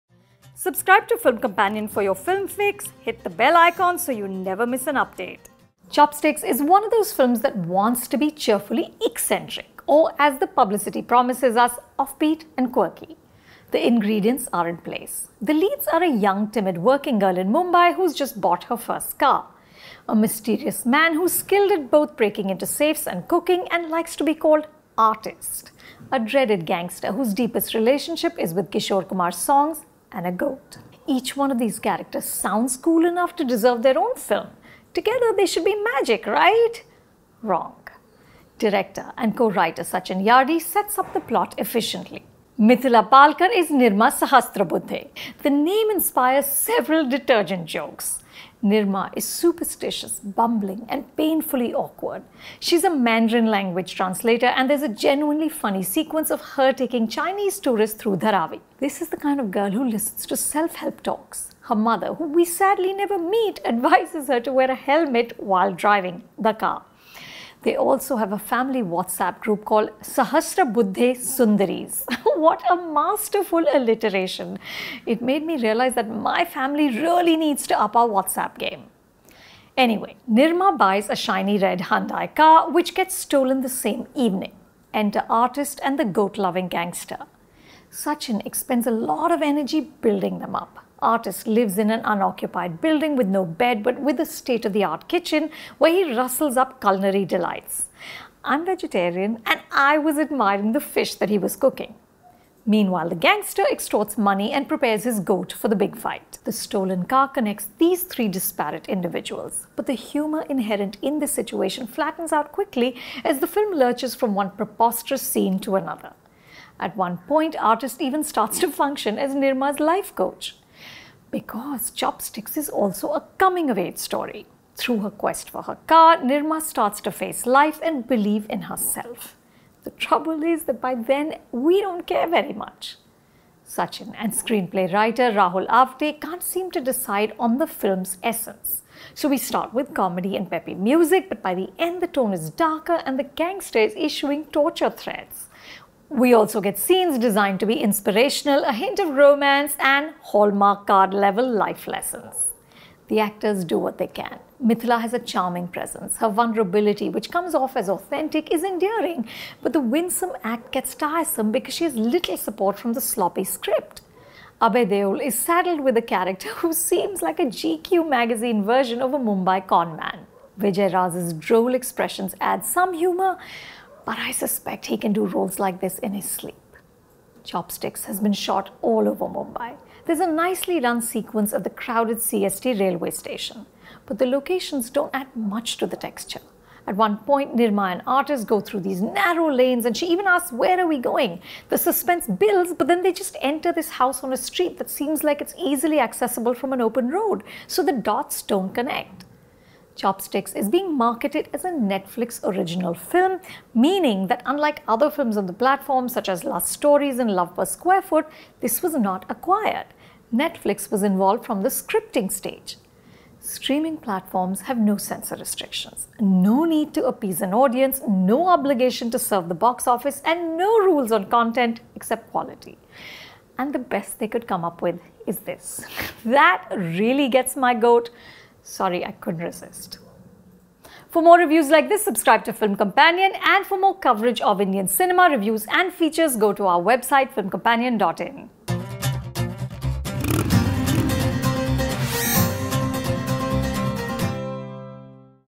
Chopsticks Movie Review by Anupama Chopra | Abhay Deol | Mithila Palkar | Film Companion
Here's Anupama Chopra's review of the Netflix original film 'Chopsticks', starring Abhay Deol, Mithila Palkar, Vijay Raaz and a goat.